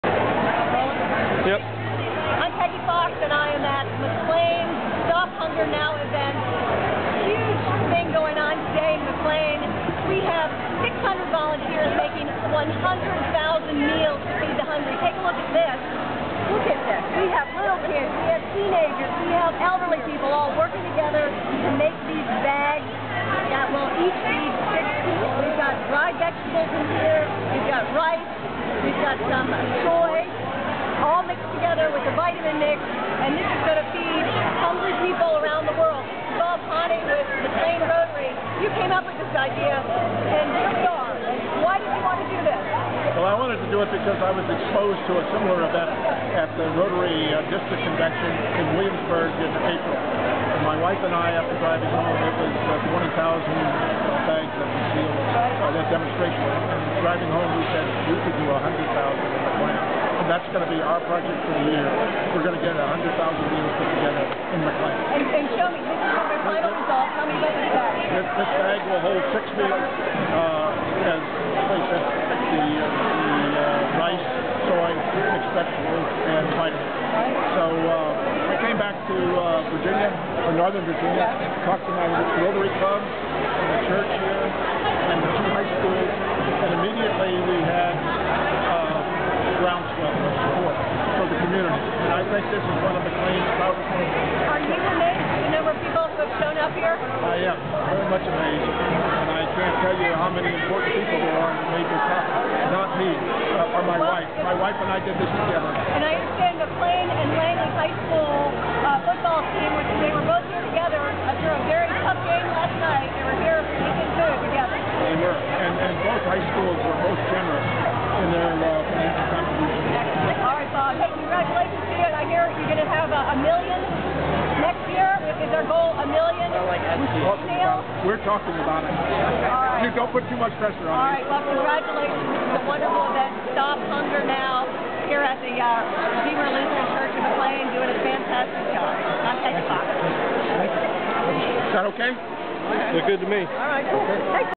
Stop Hunger Now; McLean, VA
More than 600 volunteers in McLean, Virginia came out Nov. 6, 2010 to pack 100,000 meals for the hungry around the world.   Next year: 1 million!